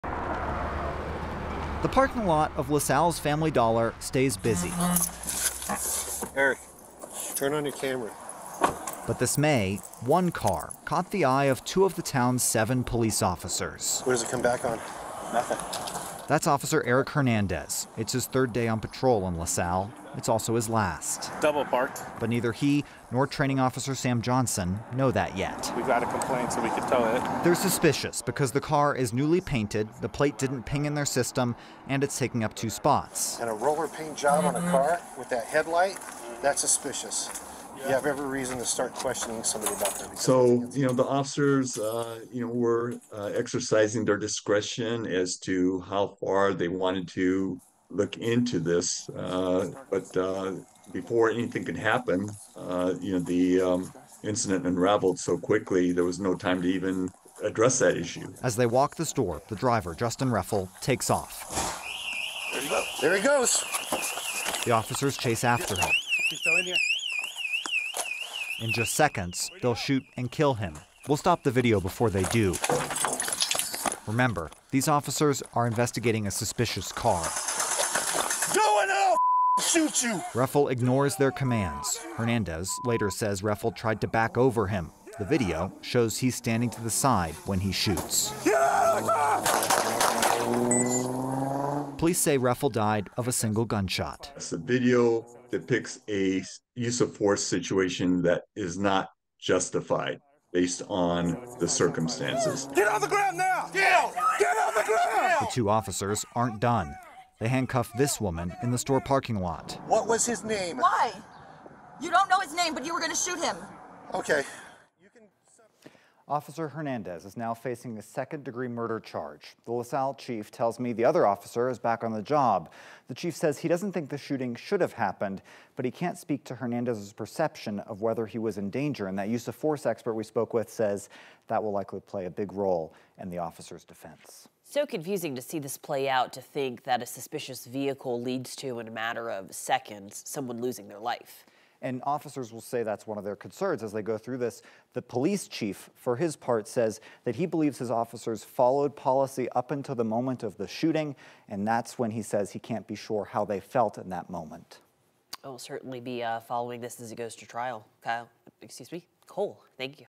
Body camera footage shows officer shooting, killing man initially contacted for double parking
LaSalle Officer Erik Hernandez faces a second-degree murder charge for fatally shooting a man who was initially contacted for a double-parked car.

9NEWS (KUSA) is located in Denver, Colorado.